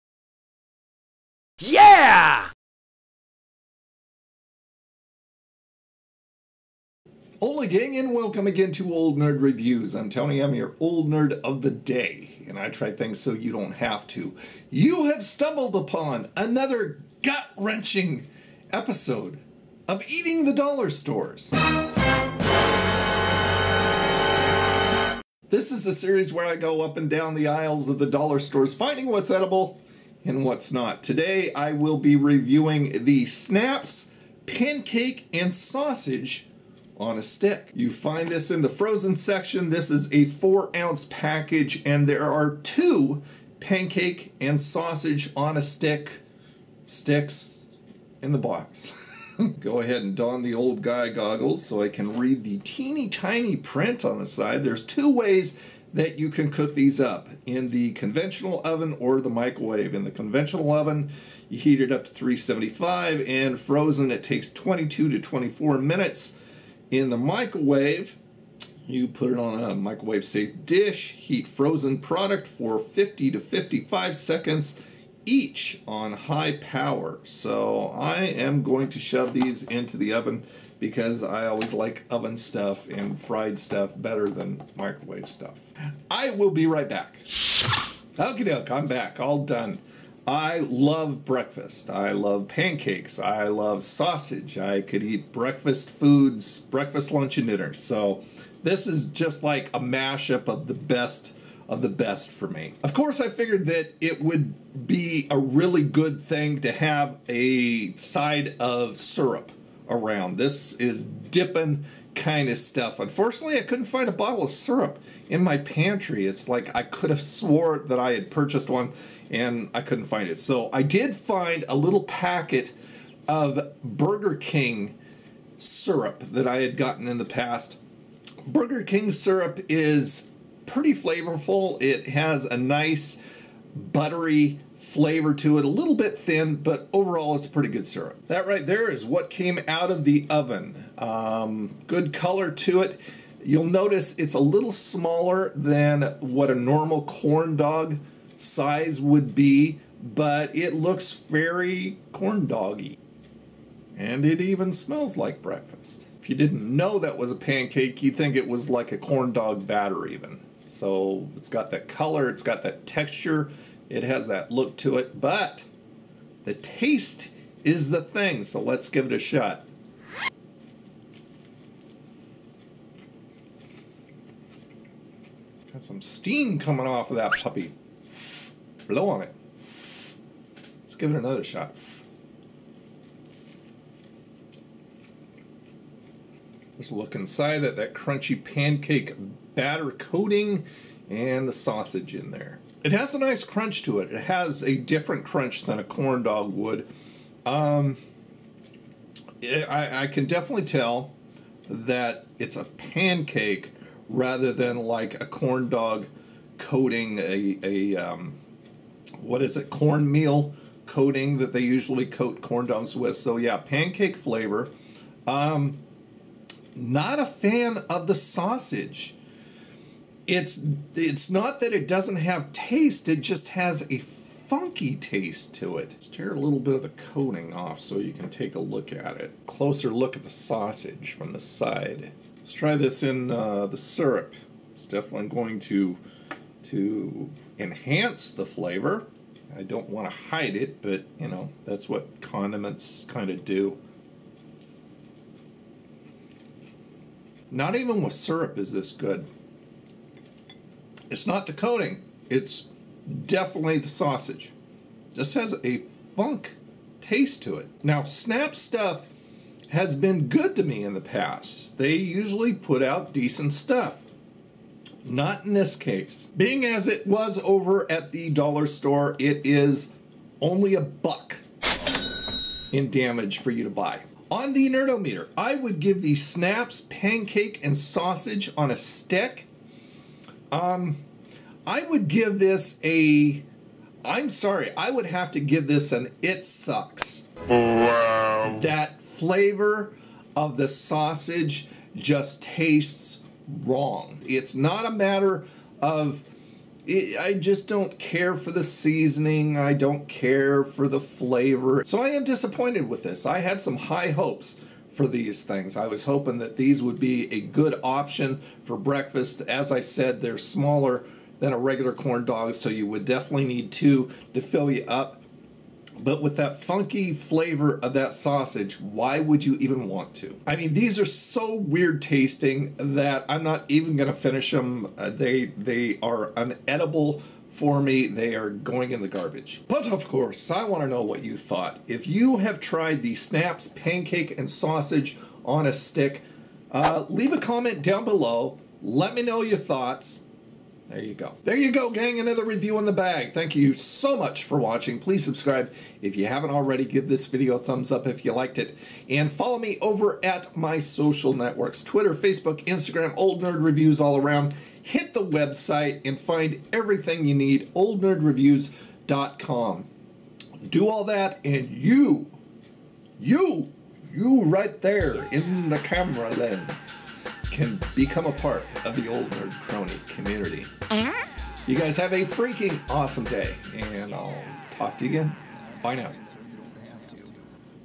BREAKFAST ON A STEEK! Snapps Pancake & Sausage On A Stick REVIEW 💵💲
Hoping for a tasty breakfast as a review the Snapps Pancake & Sausage On A Stick. Just another gut-wrenching episode of Eating The Dollar Stores.

pancake and sausage on a stick price,sausage pancake dippers